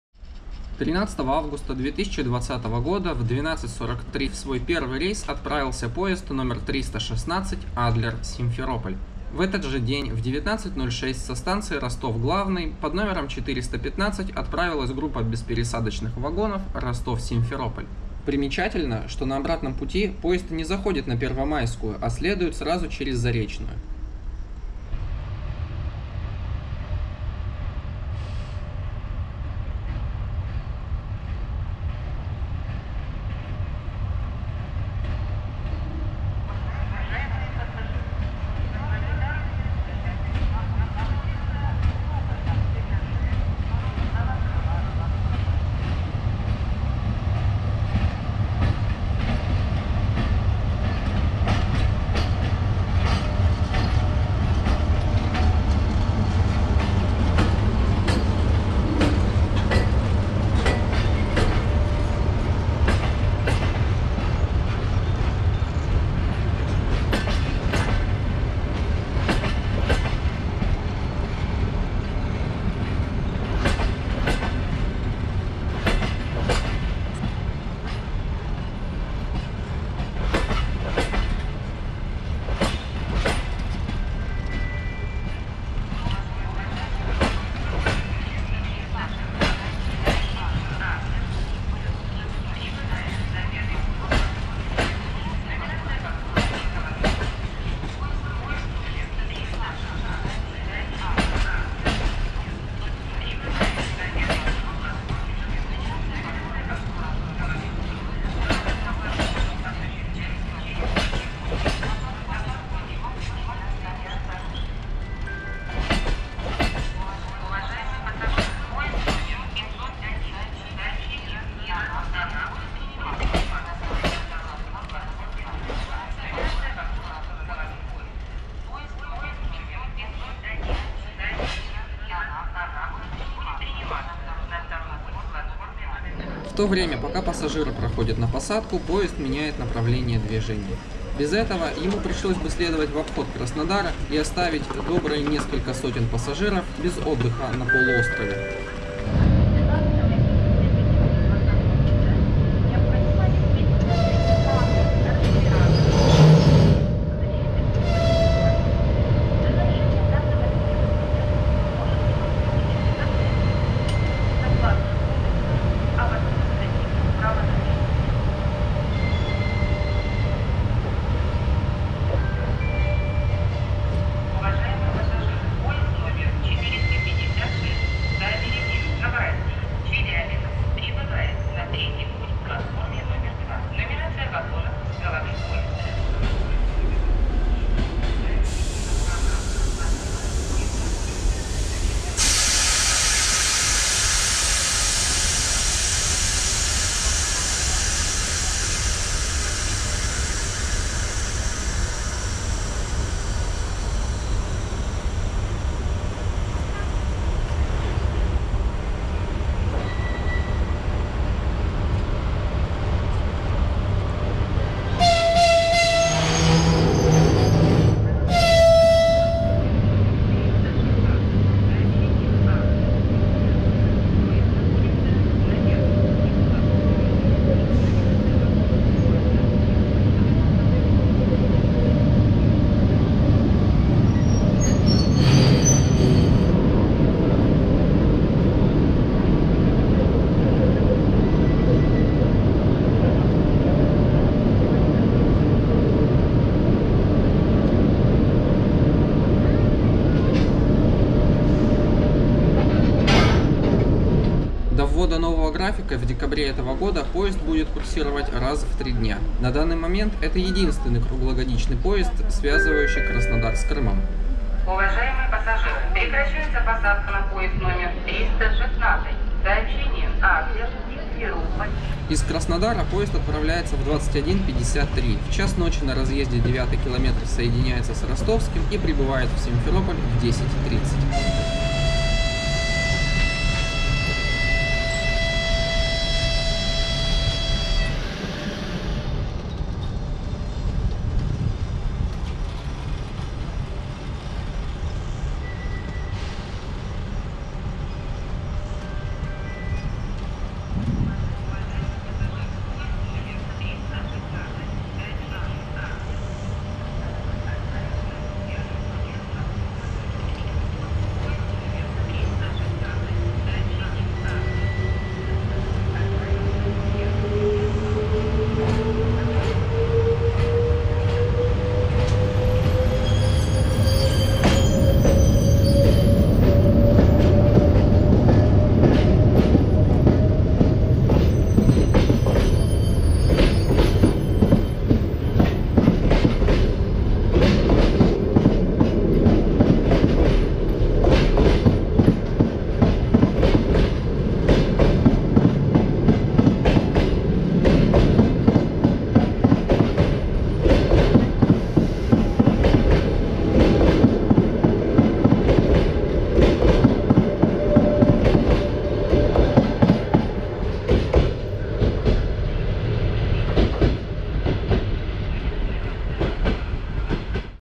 THE FIRST TRAIN ADLER SIMFEROPOL IN NIGHT KRASNODAR! EXCLUSIVE! TRAIN OVERVIEW
Review of the very first train from Adler to Crimea! Arrival, parking, change of direction, coupling and uncoupling of an electric locomotive, departure.
As well as an overview of the route and schedule!
P.S. Like the Kislovodsk train, train 315/316 runs every 3 days
13 August 2020, 21:05 - 21:55

Provided by the channel: Ilya Sakharov